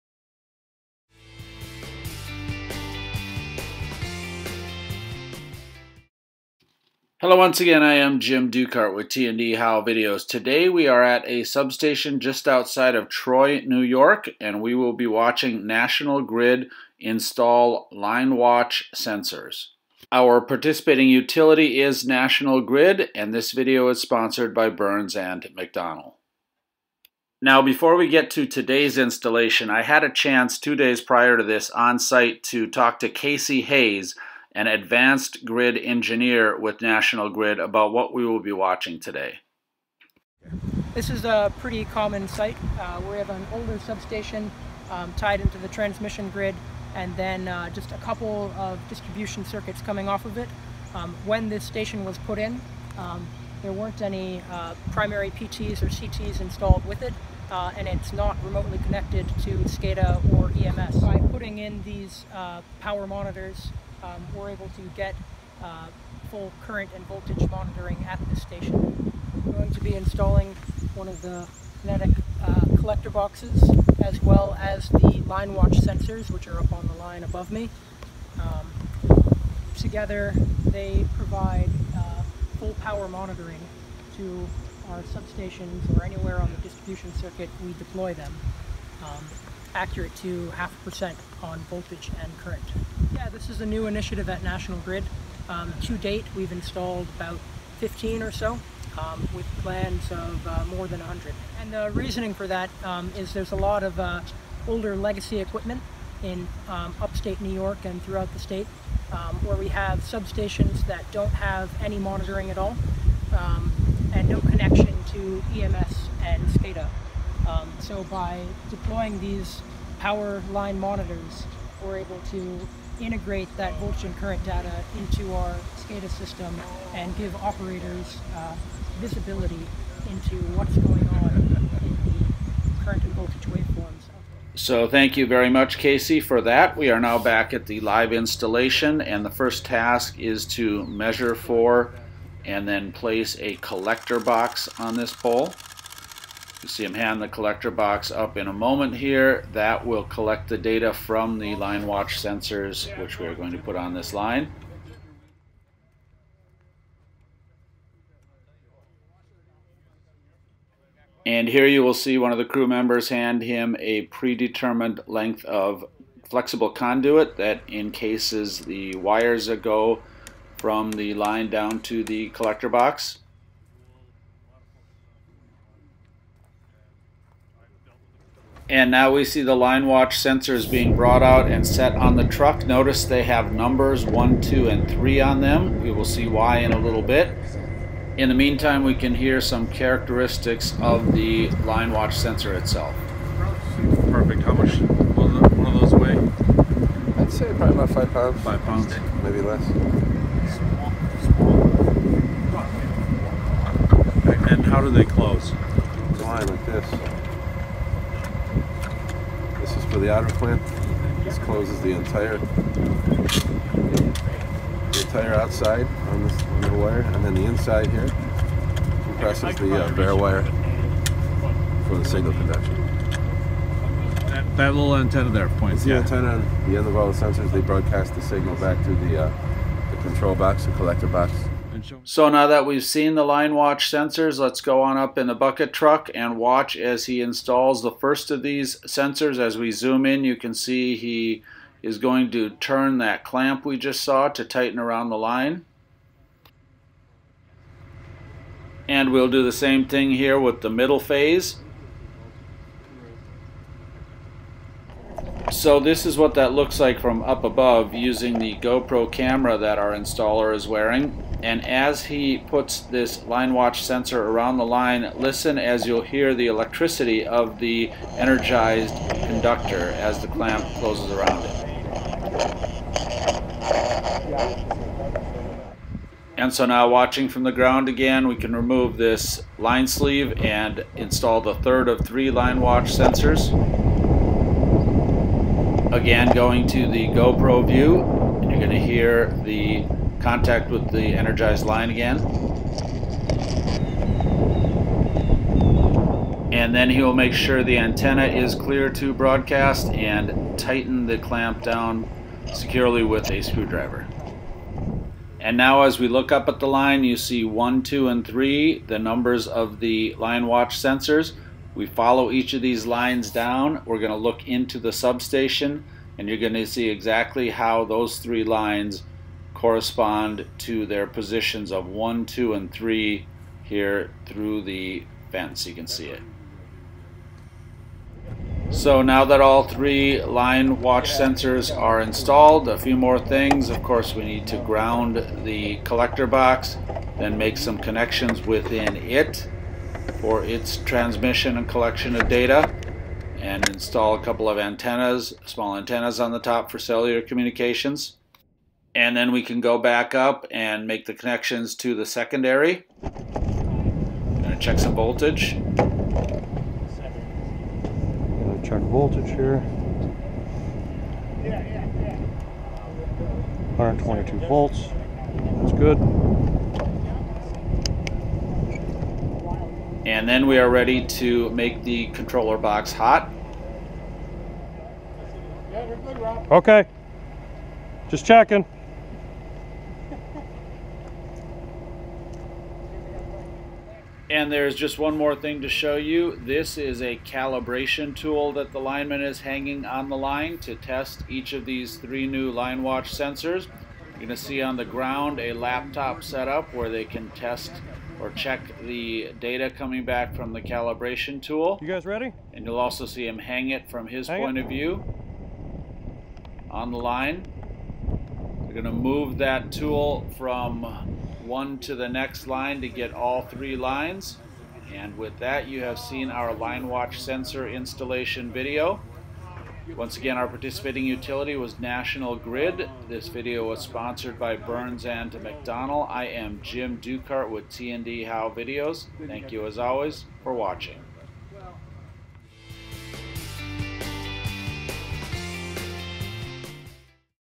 Line Watch Sensor Installation
Near Troy, New York, National Grid shows the installation of Line Watch Sensors at a substation.